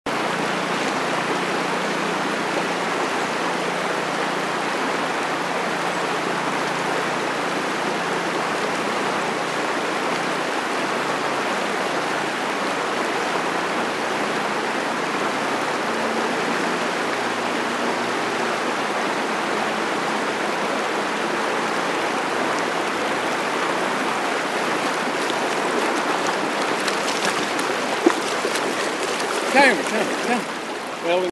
Paddling cypress creek with three dogs
We ran into TimesDaily videographer Brandon Ross at this bend on the river on Cypress Creek back in August.  Riding with me is our golden retriever, Mattie.  The two swimming are two rescues, Havana and Kramer.  The timing of this video is special, because, we found that Kramer had a tumor later in the summer. I'm guessing  he's now up in big creek in the sky.